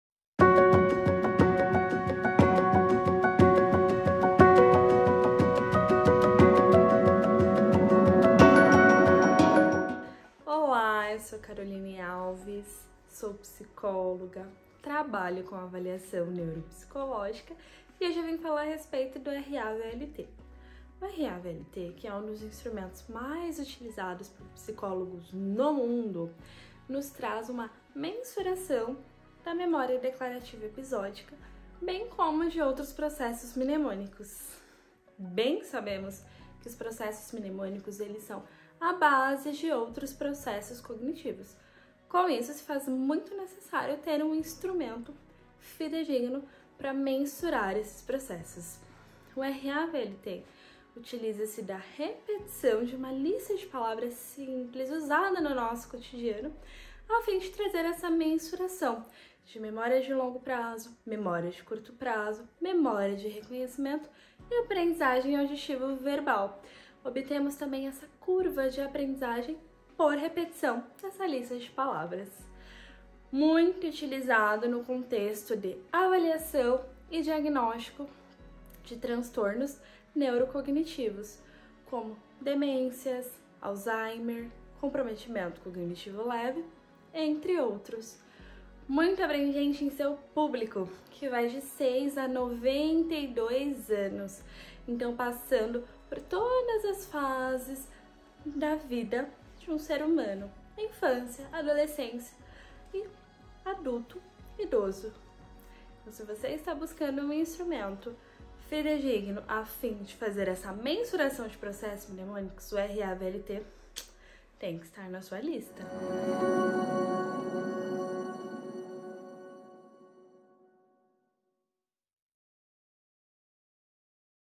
Aprendizagem auditivo-verbal | Vetor Editora
A psicóloga Caroline Alves, trabalha com avaliação neuropsicológica e, neste vídeo, ela traz mais detalhes sobre o instrumento RAVLT, o qual permite mensurar a memória declarativa episódica, bem como de outros processos mnemônicos.

Vetor Editora, Presente Na Sua Vida.